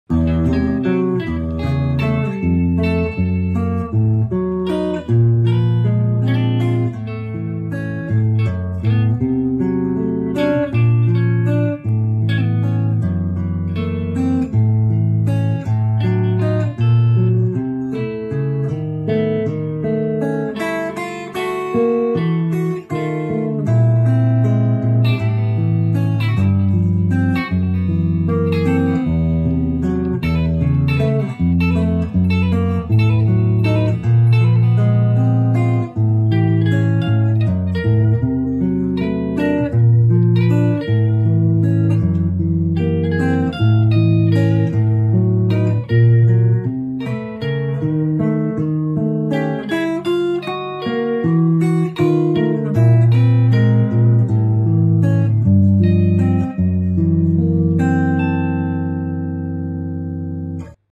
Agam Hamzah's Tantangan Riff
Agam Hamzah chord melody
Arlo Hennings solo improvisation